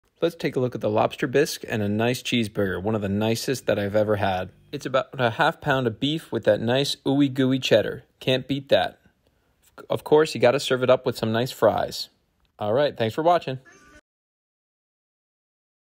Monster Gourmet Cheeseburger and Lobster Bisque!
Look at this amazing half pound cheeseburger and bowl of lobster bisque soup.  Really tasty.